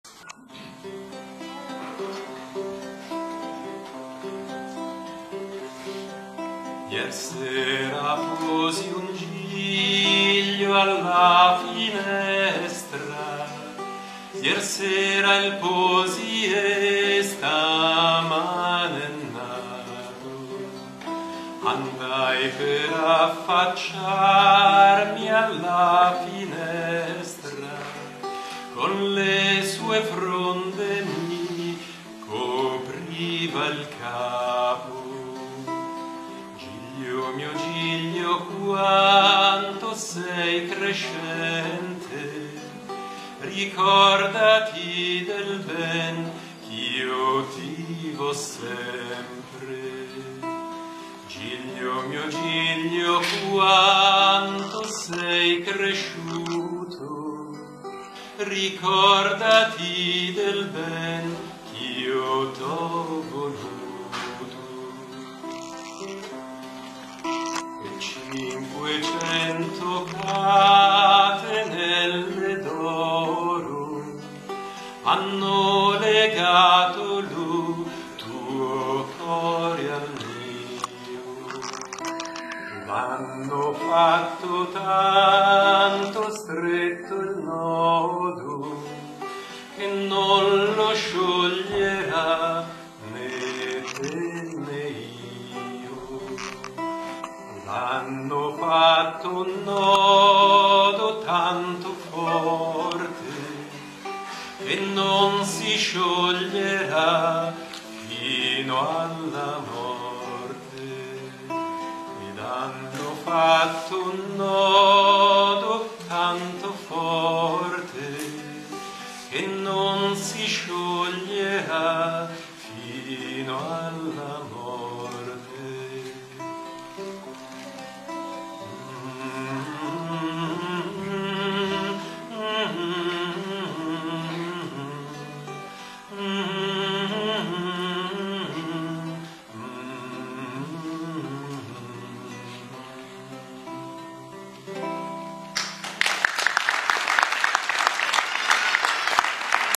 Le cinquecento catenelle
Volpiano (TO), 16 novembre 2019, in occasione della decima edizione del Premio letterario nazionale Enrico Furlini.
Un estratto dell'intervento sugli strumenti musicali antichi.